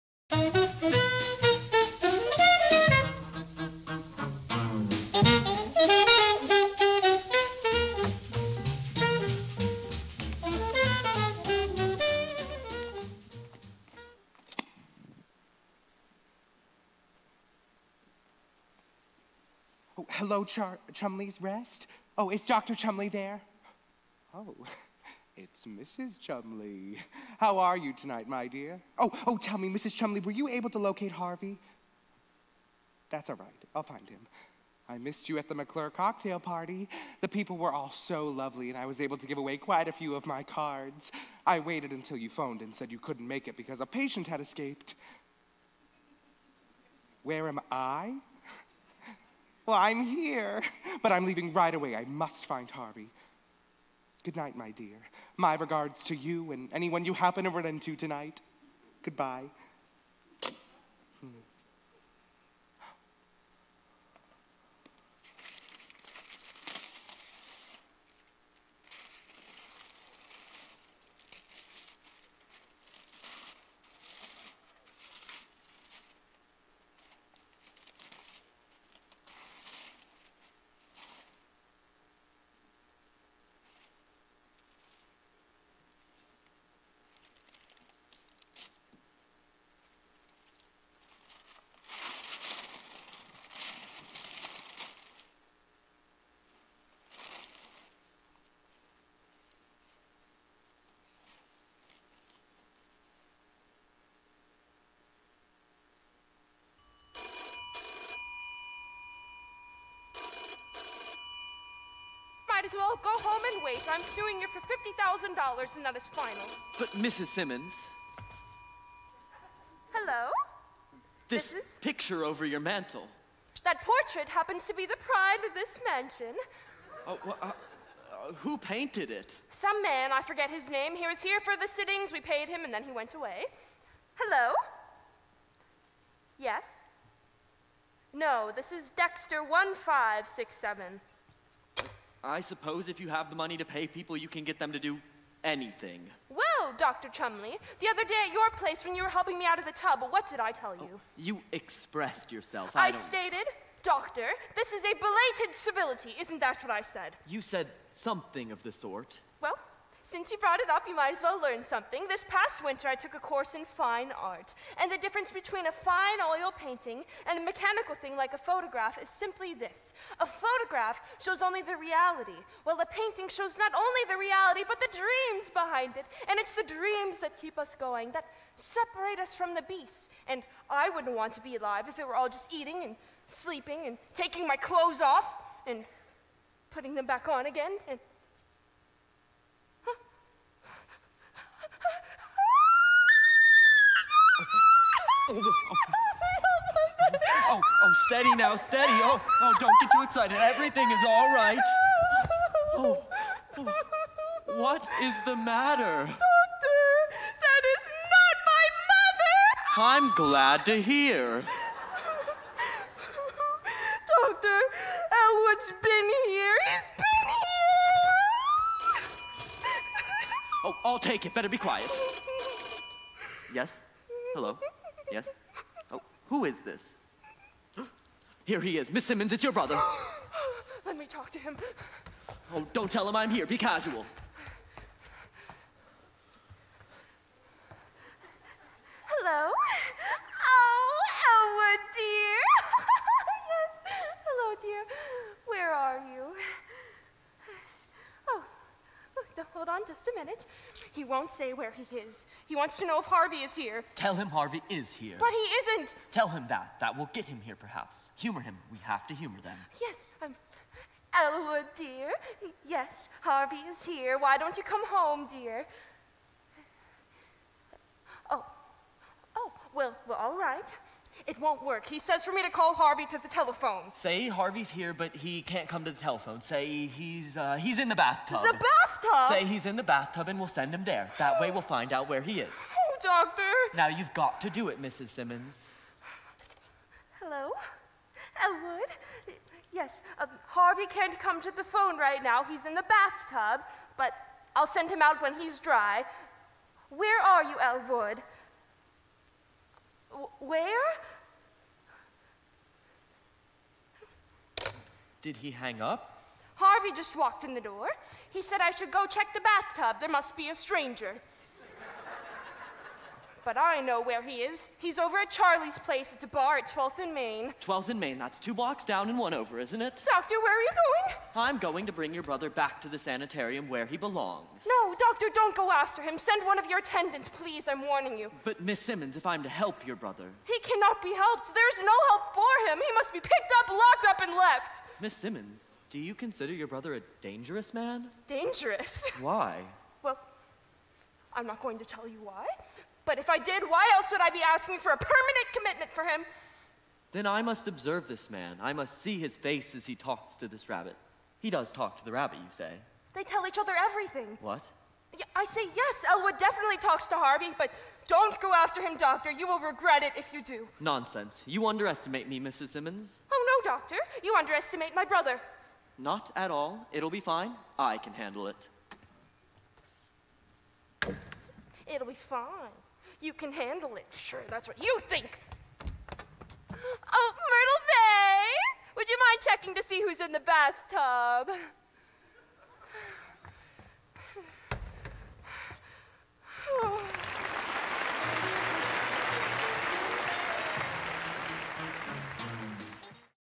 Stivers School for the Arts Production Tribute: "Harvey" MVHSTA May 05.19.20
If you’re just joining us, it’s MVHSTA MAY! Today, we are featuring the work of Stivers School for the Arts' theatre program! Take a look at this video from their production of "Harvey" that showcases the caliber of talent right here in the Miami Valley!